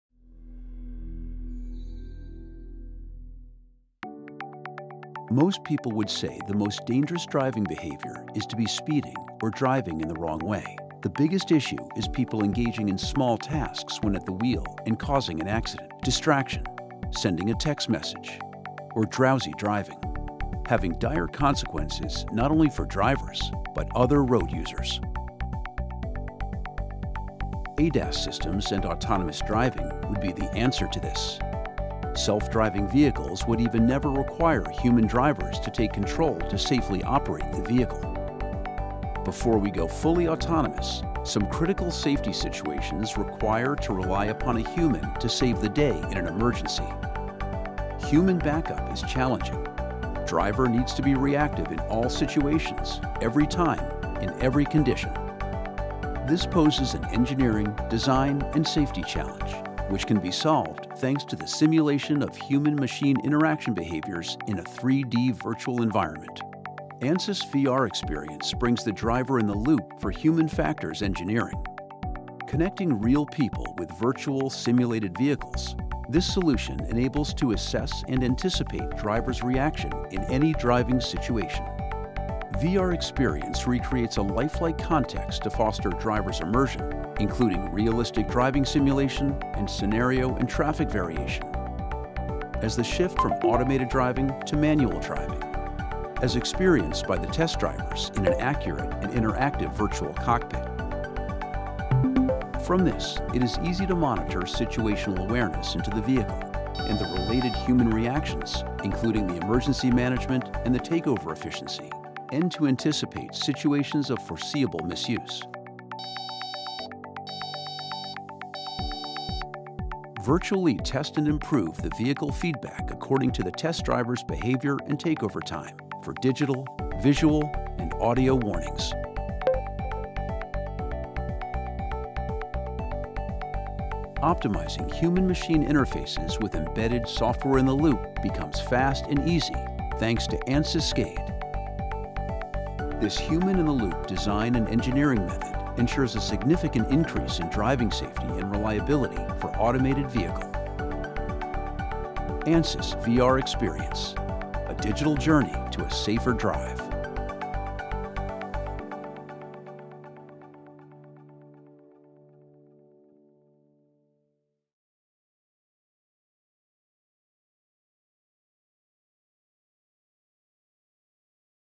A Digital Journey to a Safer Drive with Ansys VRXPERIENCE
Ansys VRXPERIENCE simulation places humans in the loop for the design and testing of ADAS vehicles.
By simulating human–machine interactions, VRXPERIENCE enables automotive OEMs and suppliers to assess and anticipate drivers’ reactions under myriad virtual conditions. This video demonstrates VRXPERIENCE in action — from behind the wheel in a virtual cockpit.